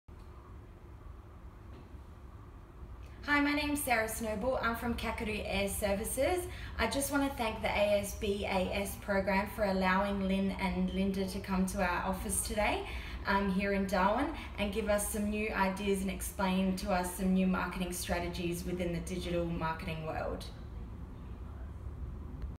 Digital Express Testimonial from Kakadu Air
Hear Ngaire from Kakadu Air share her fantastic experience with the Digital Express Program recently.